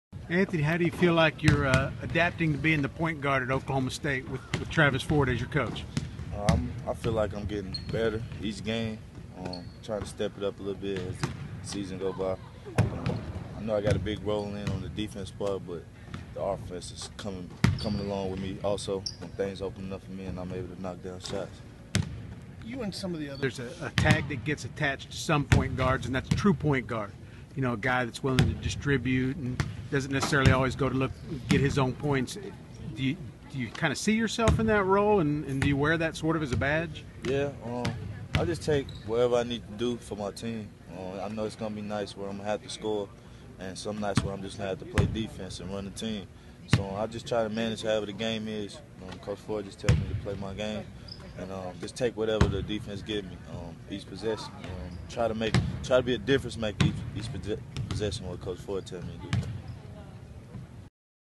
OSU basketball: Hickey talks offense and defense (2014-12-13)
Anthony Hickey talks about being OSU's point guard.